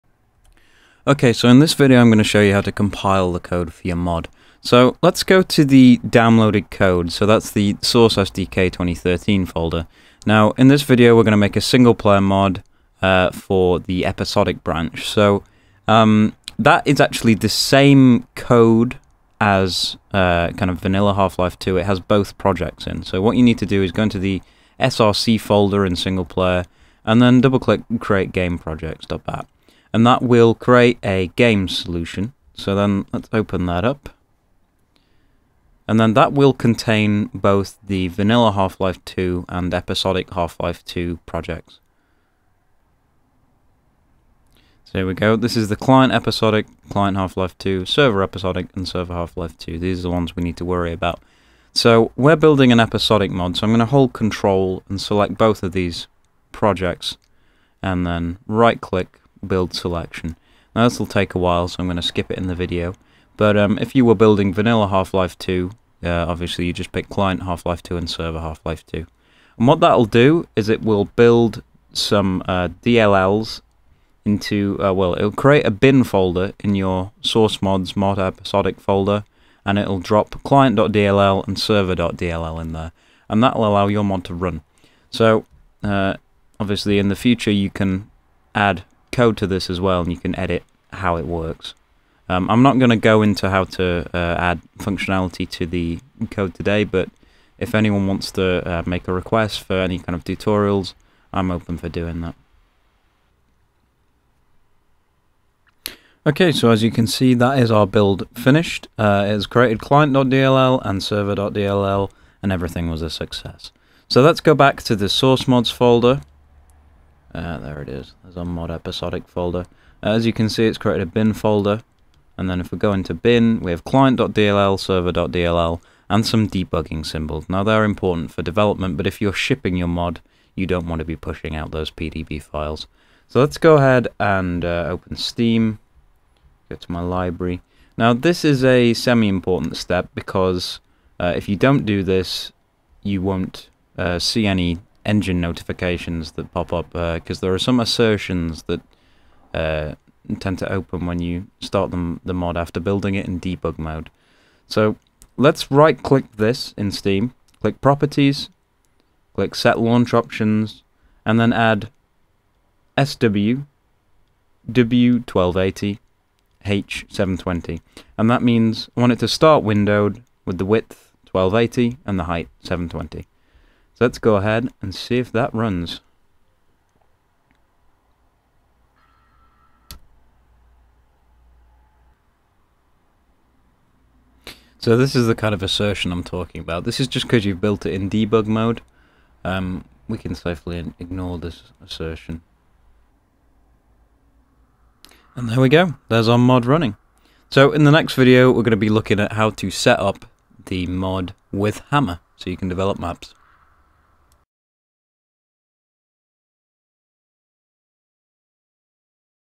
Source SDK 2013 Tutorials: 2 - Compiling the C++
A tutorial about how to compile the code for Source SDK 2013. Slides